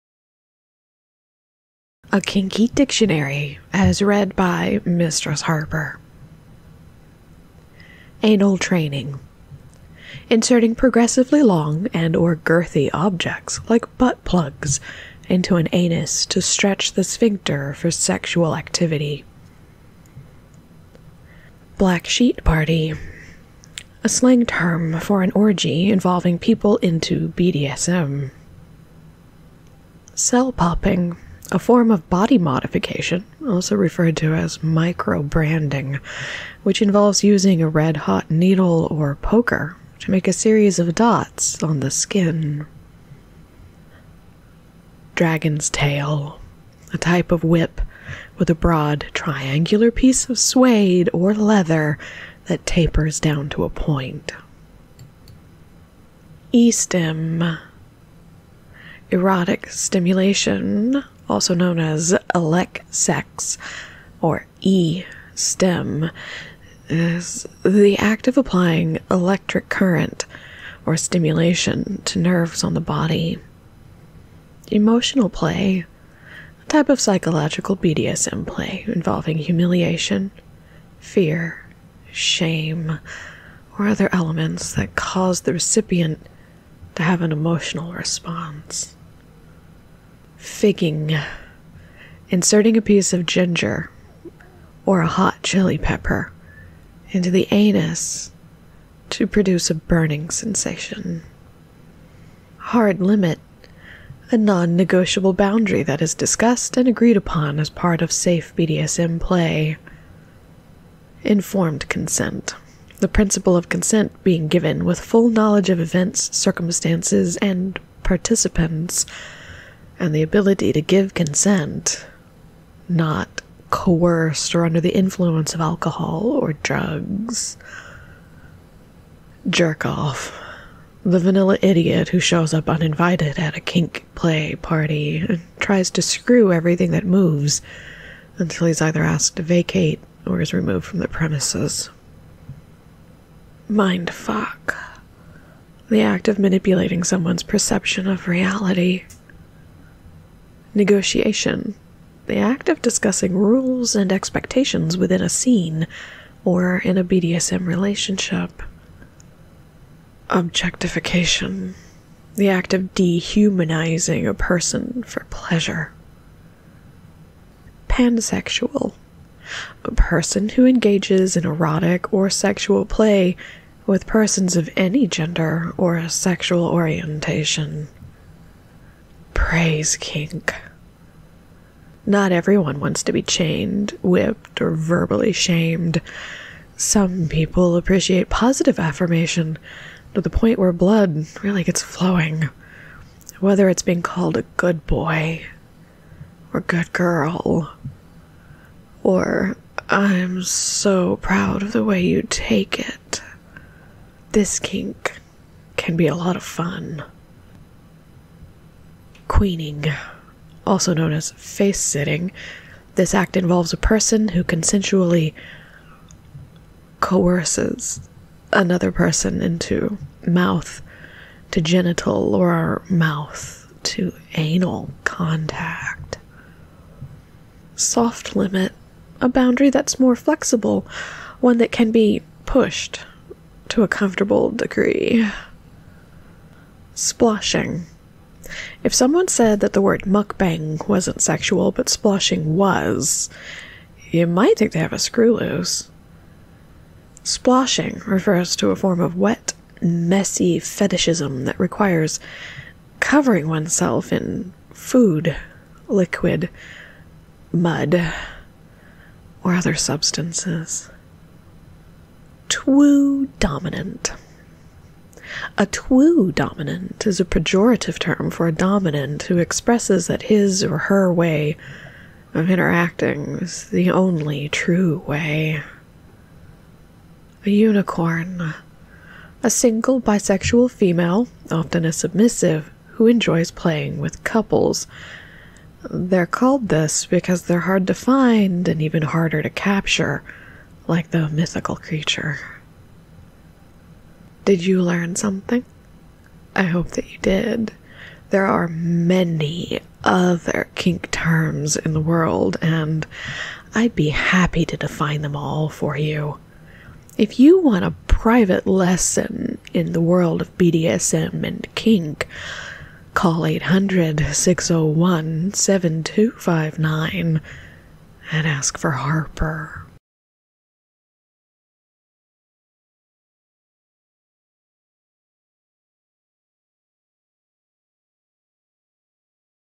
Harper Reads a Kinky Dictionary
I've been told that I could read a dictionary and make it sexy, so I gave it a try!